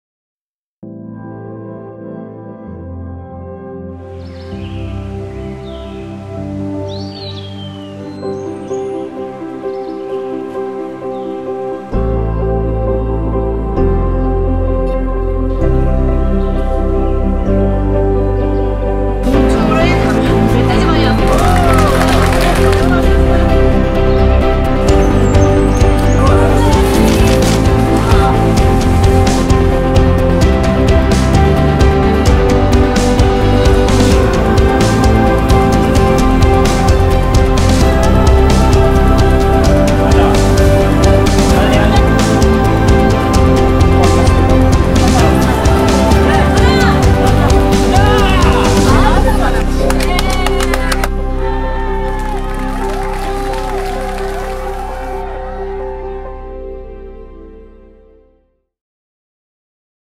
[제3회 2019 파타고니아 볼더러스 데이(Patagonia Boulderers Day): 스케치 필름]
[제3회 2019 파타고니아 볼더러스 데이: 스케치 필름]

지난 5월 25일, 약 200명의 볼더러가 광주 무등산 선비 바위 일대에 모였습니다.

제3회 2019 파타고니아 볼더러스 데이에 참여한 이들은 루트 개척 등 다양한 볼더링 프로그램을 즐겼습니다. 당시의 생생한 현장을 스케치 영상으로 만나보세요.